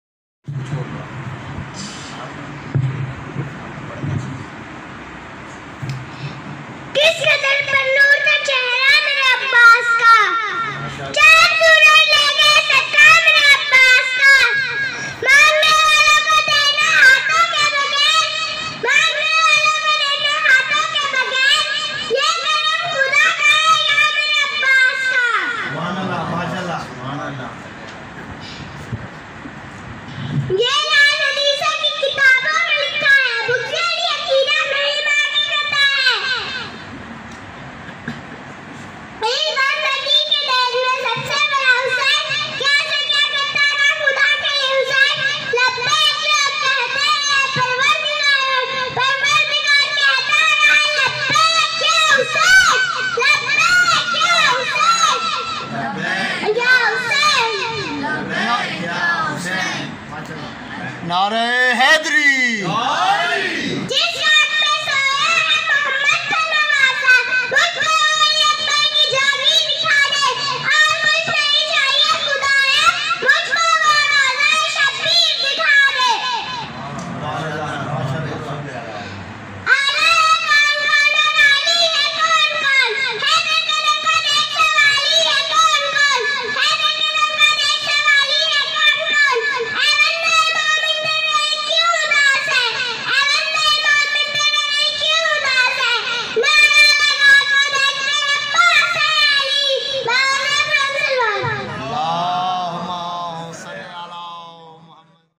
is choty bacche ko zaror sune jazakallah
momneen is chote bacche ki housla ahfzai zaror karen
moula salamat rakhe aap sabko amen